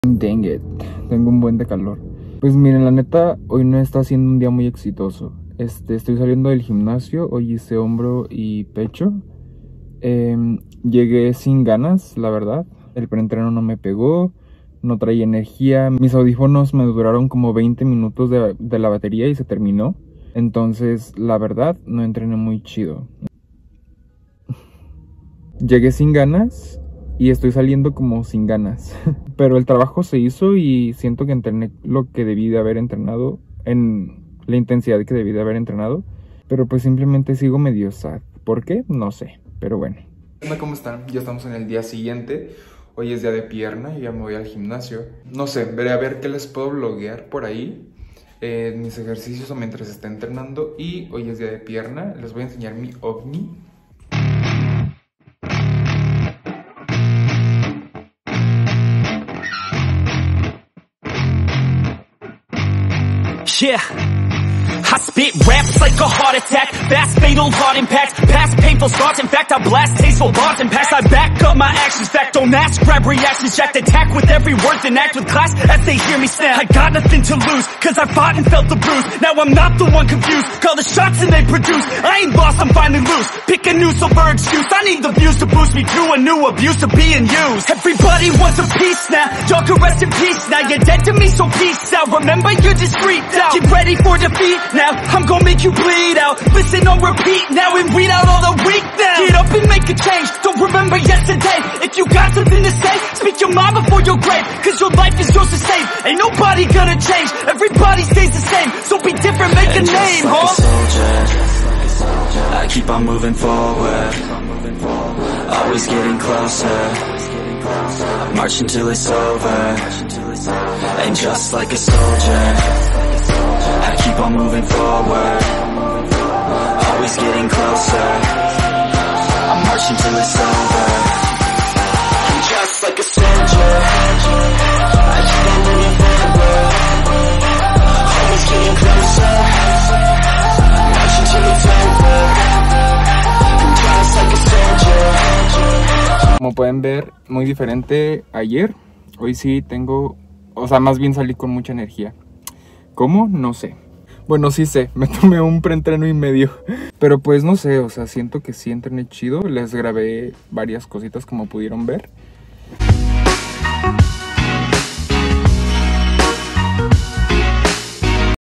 UN DÍA CONMIGO | RUTINA DE PIERNA | JOSUÉ FLORES
En este vlog me acompañas en un día en mi rutina de pierna y algunos pensamientos que se tienen al entrenar…